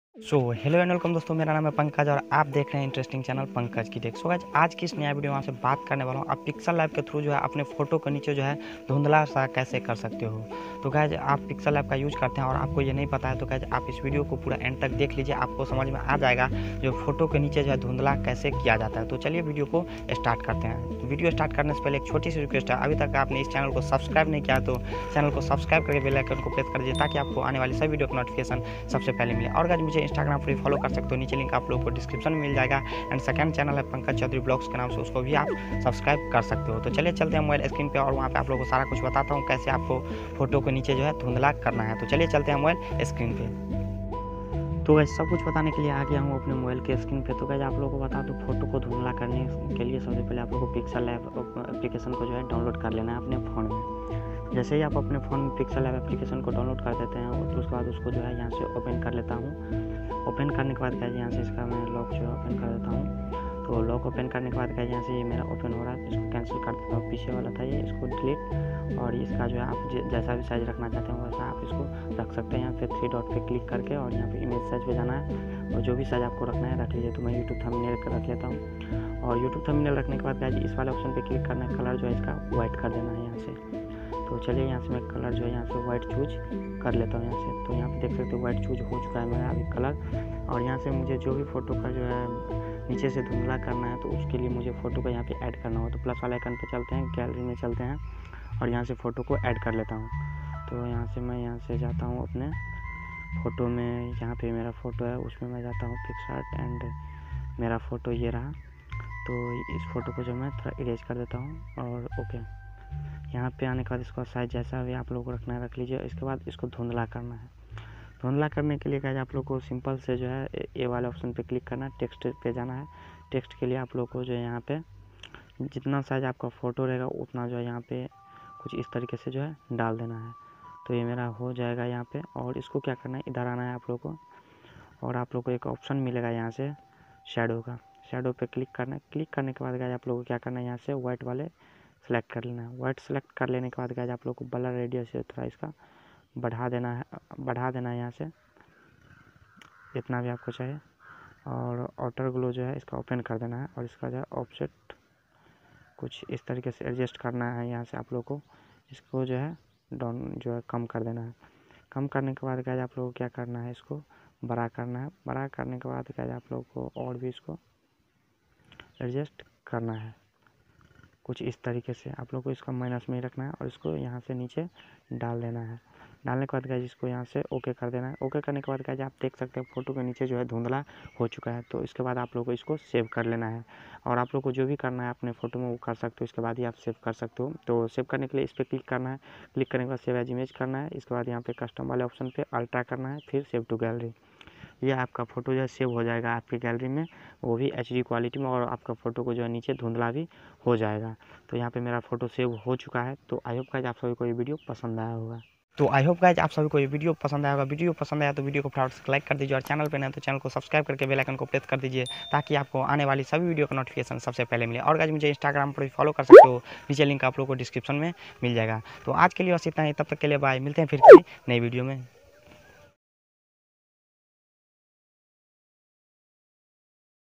फ़ोटो के नीचे धुंधला कैसे करे || photo ke niche dhundhla kaise kare || pixel lab se photo ko dhundhla
Hii friends

Dosto aaj ki iss video me maine aapse bat ki hai pixel lab ka use karke photo ke niche dhundhla kaise kare

I Hope aap sabhi ko ye video pasand aaya hoga

Pankaj chaudhary From-Madhuabni,Bihar (india)